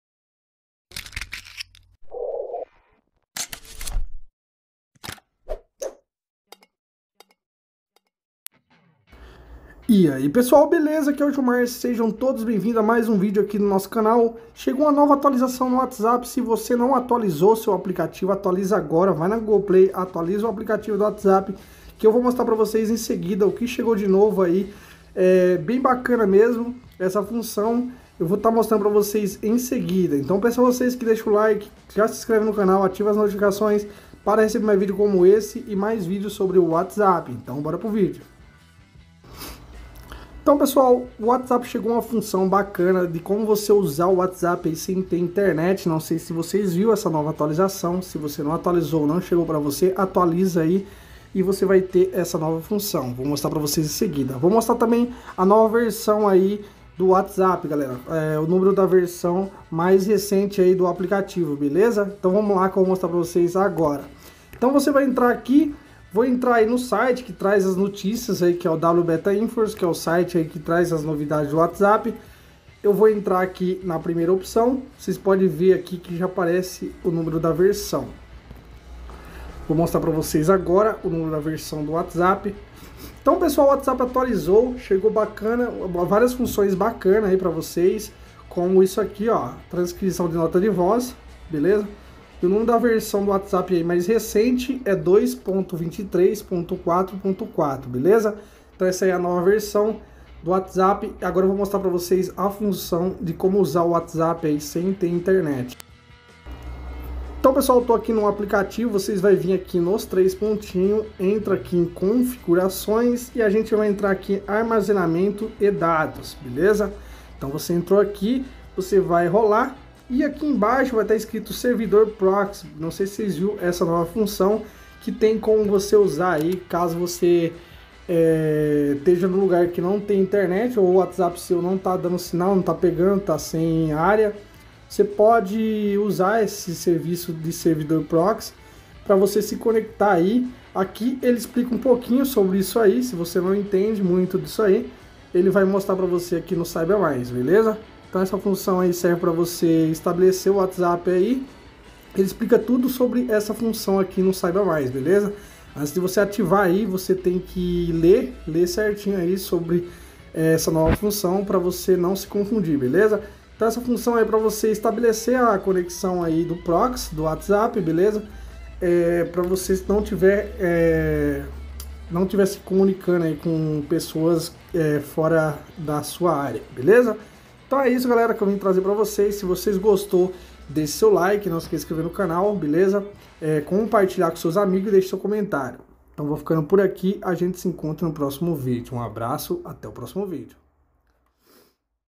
Nova atualização do WhatsApp!! Permite enviar mensagens sem internet
Atualização do WhatsApp permite enviar mensagens sem internet.

Você viu? Chegou nova atualização do aplicativo que permite você se conectar quando aplicativos estiver sem conexão.